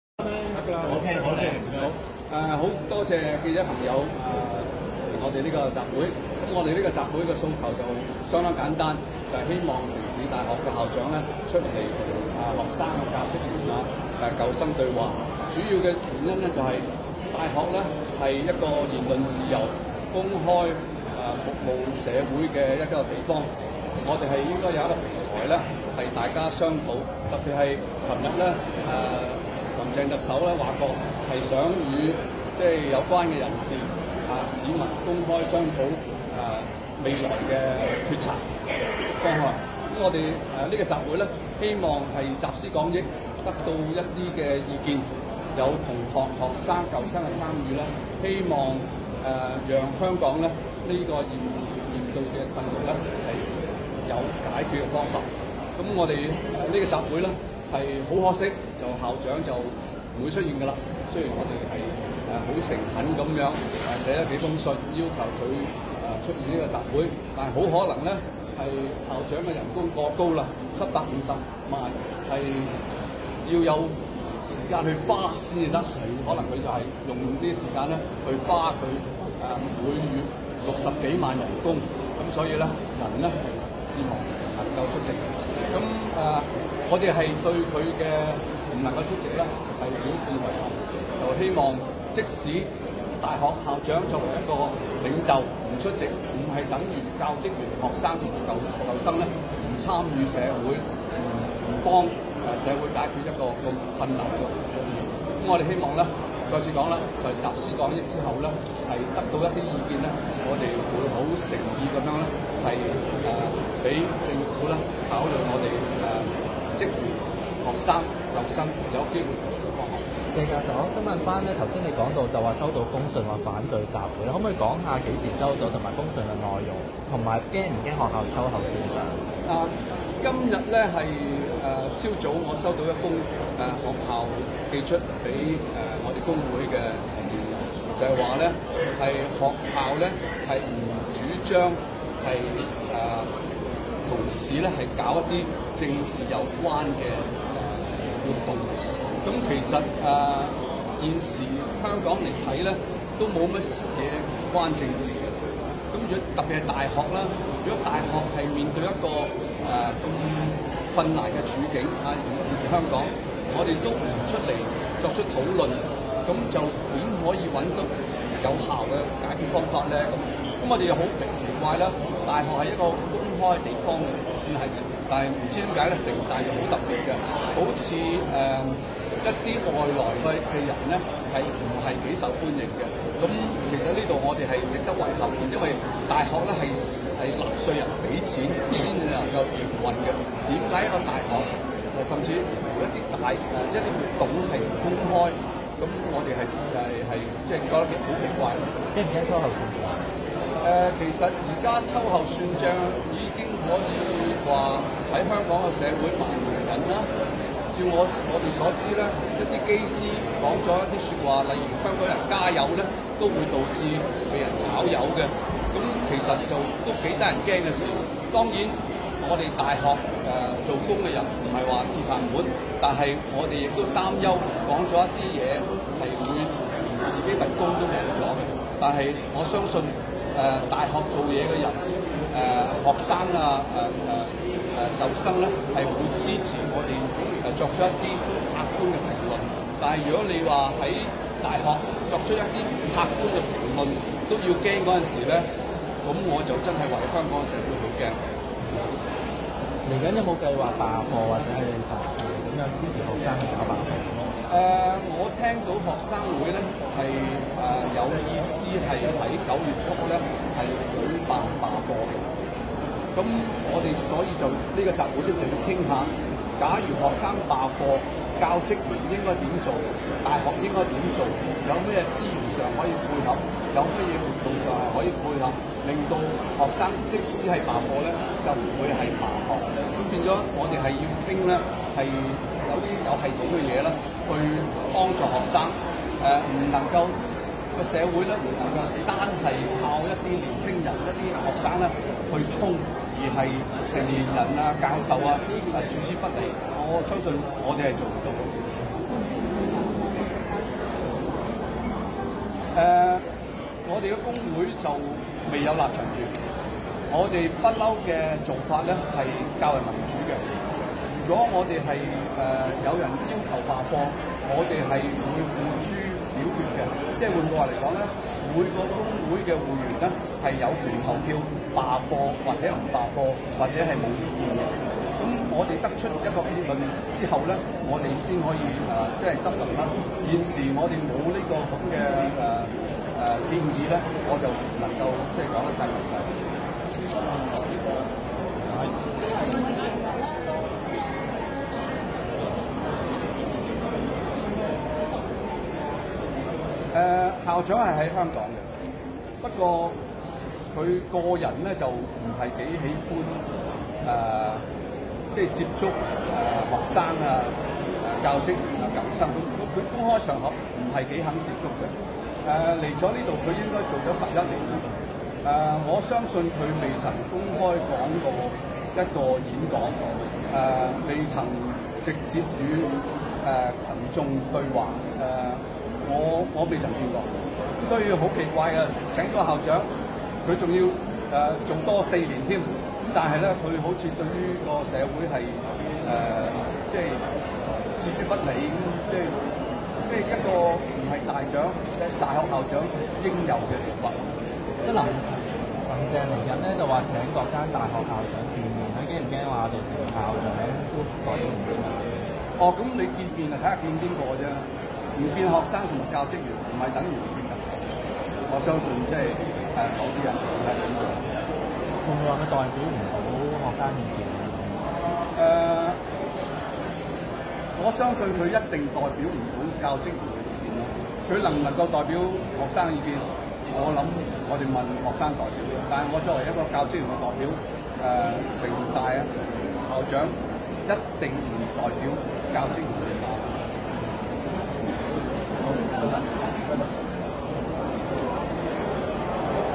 【要求與城大校長郭位對話】
13:00 香港城市大學【城大速報 - 同學各位找郭位】
前香港民主黨成員、現任城市大學教職員協會主席謝永齡原出席今日城大「守護吾城」集會，但可惜城大校長郭位明確拒絕會面，集會終告取消。一眾教授和學生均到場表態，昐校長早日聽見學生的聲音。
緊貼校園及社會即時新聞 @cityusu.edb